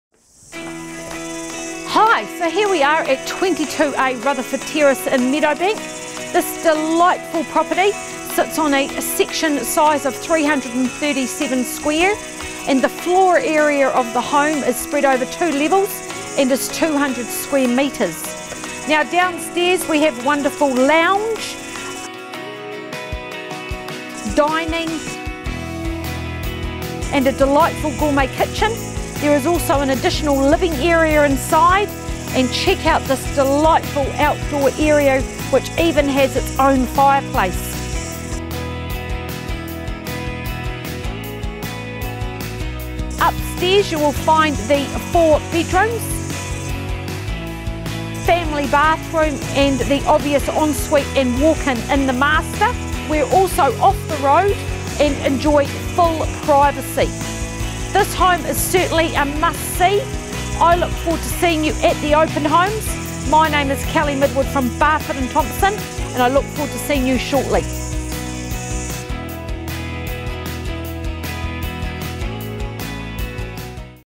SOLD - 22A Rutherford Tce, Meadowbank - Kelly Midwood
URGENT NOTICE - EARLY AUCTION

An offer has been made on this property and it is now selling at an early Auction at 10.00am Friday 20 March at City Rooms 34 Shortland Street, Auckland.  The opening bid will be the reserve and ...

Property Marketed by: Kelly Midwood, St Heliers